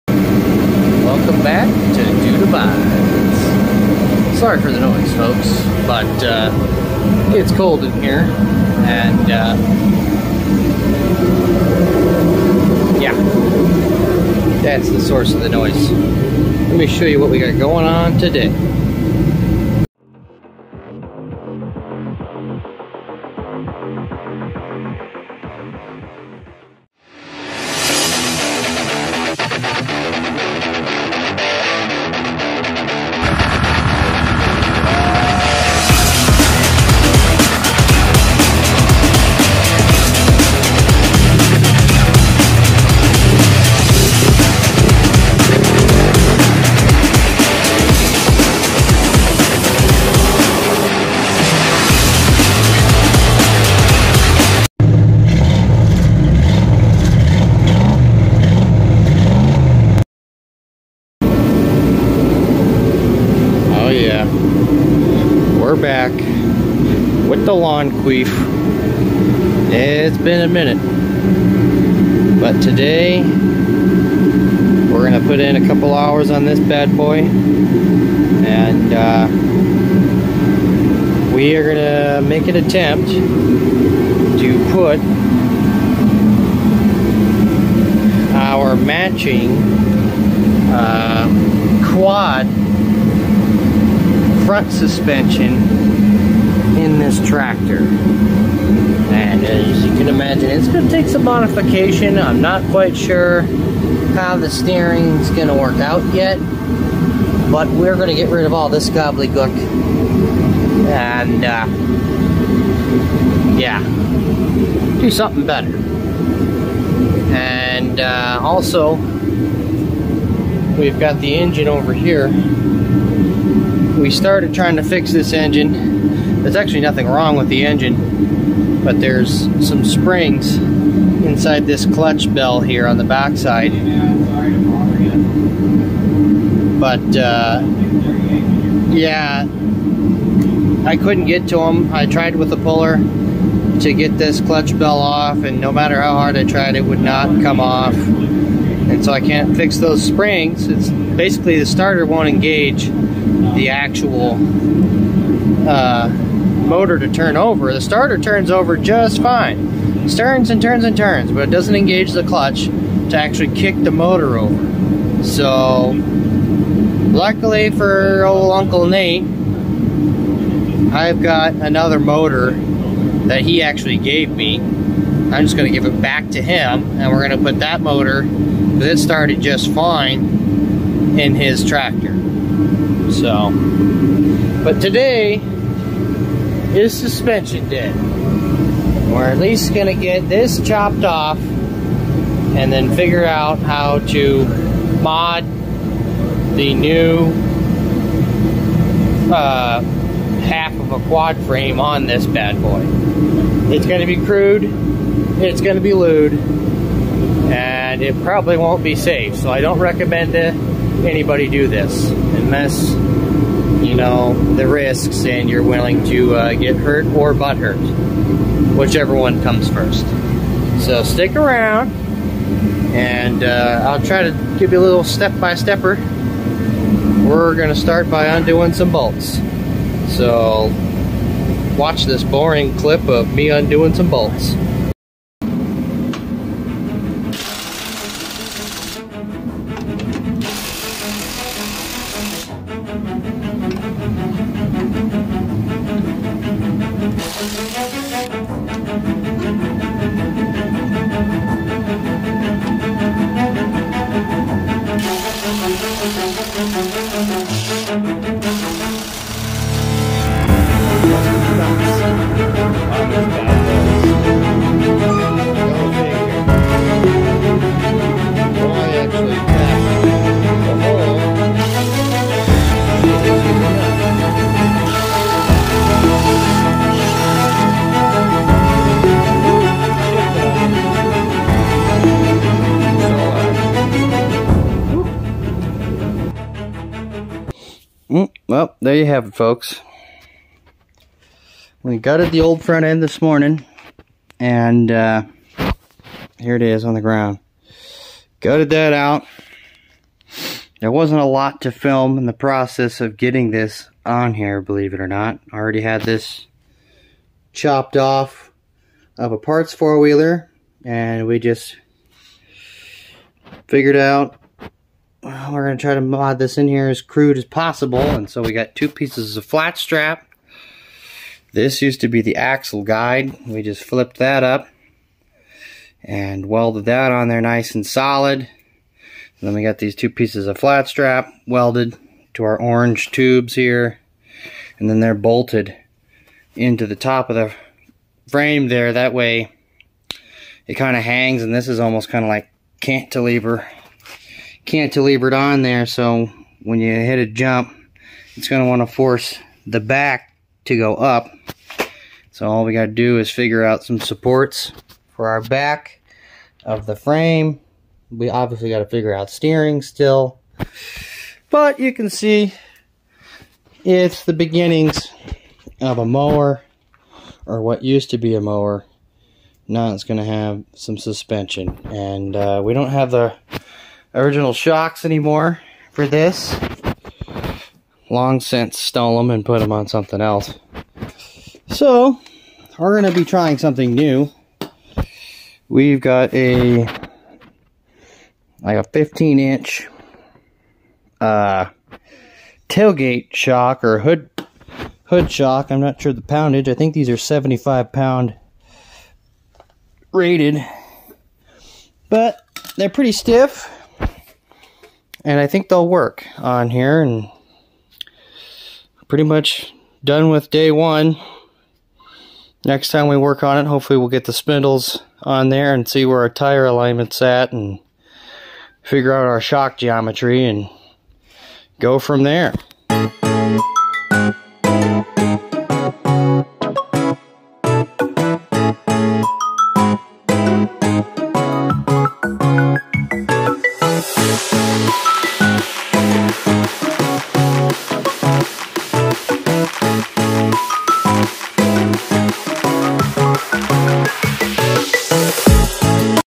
mud mower front suspension part 1 (lawn queef 545 hydro)
This is the beginning of a series that crudely shows how to modify a china quad front end on to a riding lawn mower. here are some other channels that do similar stuff. @redneckcomputergeek @giverthebeans9711 @okoffroadmowers